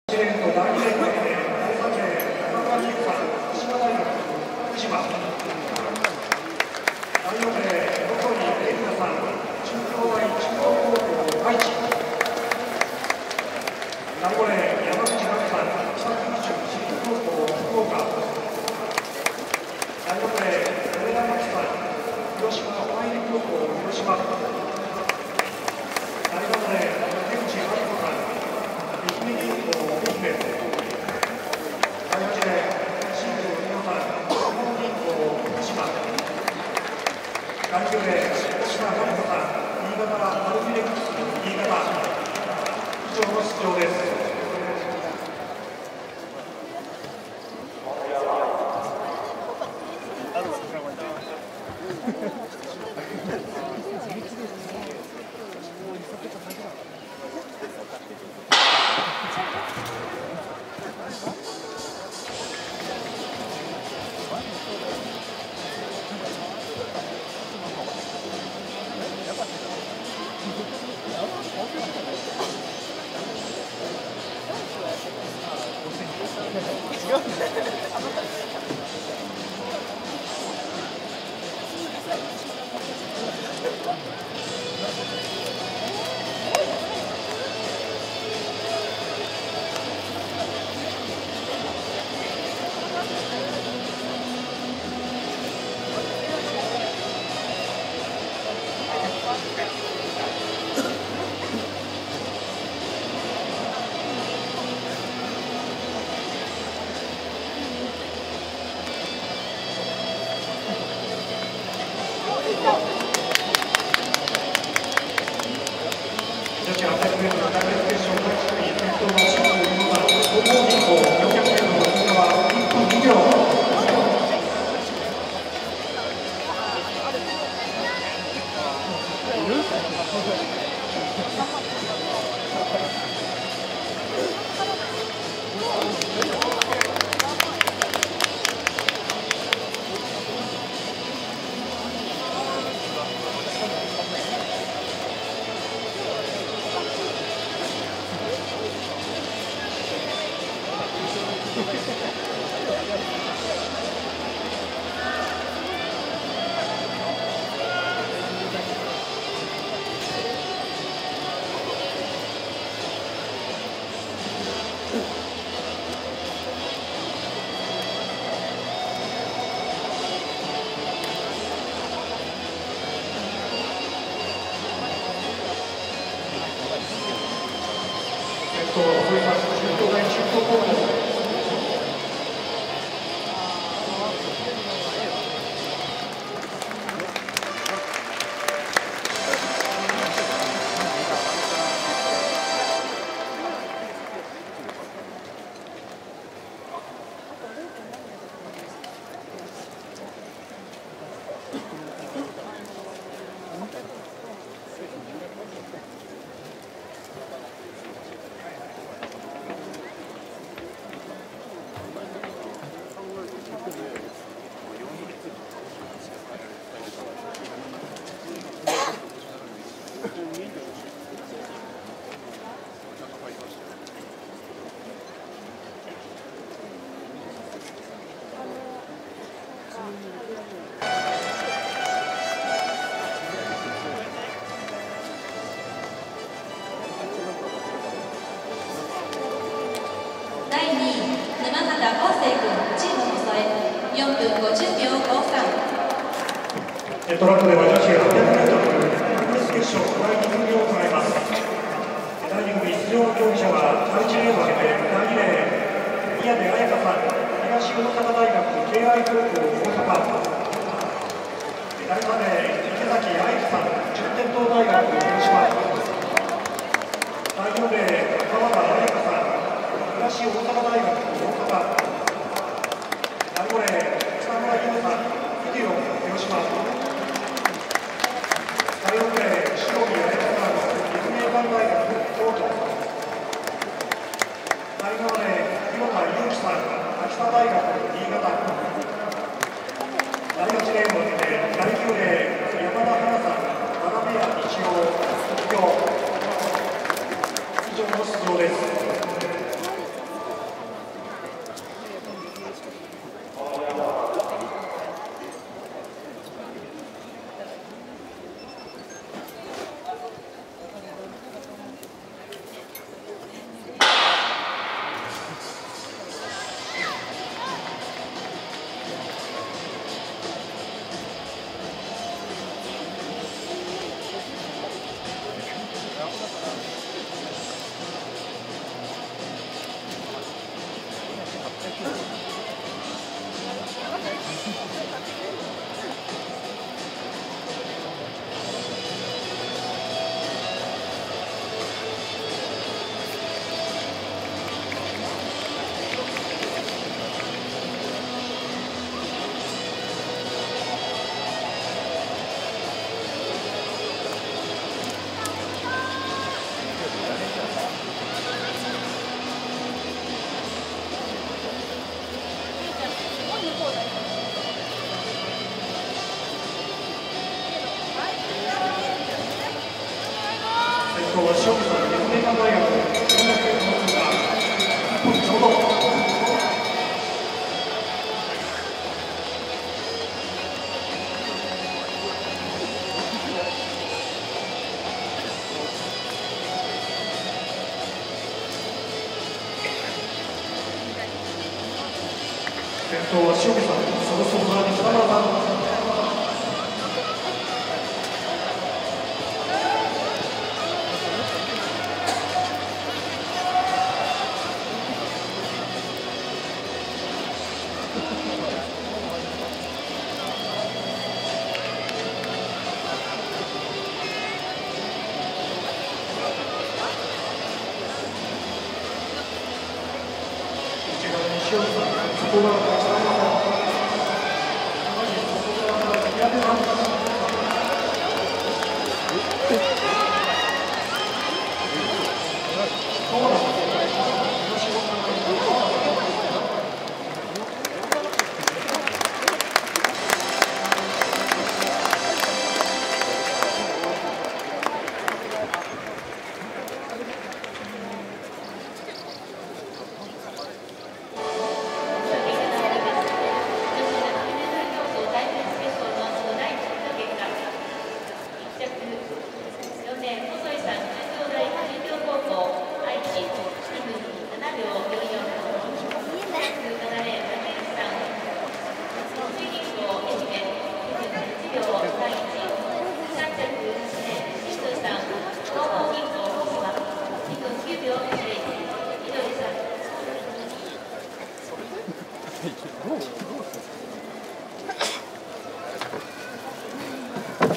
20180503 静岡国際陸上 女子800m タイムレース決勝1-2組
【1組】
1 2:07.44 細井 衿菜   中京大中京高･愛知
2 2:08.31 竹内麻里子   愛媛銀行･愛媛
3 2:09.10 新宮 美歩   東邦銀行･福島
4 2:09.40 上田 万葵   広島舟入高･広島
5 2:11.12 中村 美宇   福島大･福島
6 2:11.78 山口 真実   北九州市立高･福岡
7 2:12.15 沖田真理子   新潟ｱﾙﾋﾞﾚｯｸｽ･新潟
  欠場 須永 千尋   資生堂･東京
【2組】
1 2:02.71
大会新 川田 朱夏   東大阪大･大阪
2 2:03.36 北村 夢   ｴﾃﾞｨｵﾝ･広島
3 2:04.16 塩見 綾乃   立命館大･京都
4 2:05.81 広田 有紀   秋田大･新潟
5 2:06.40 池崎 愛里   順天堂大･広島
6 2:07.75 山田 はな   わらべや日洋･東京
7 2:11.27 宮出 彩花   東大阪大敬愛高･大阪
  欠場 大森 郁香   奥アンツーカ･東京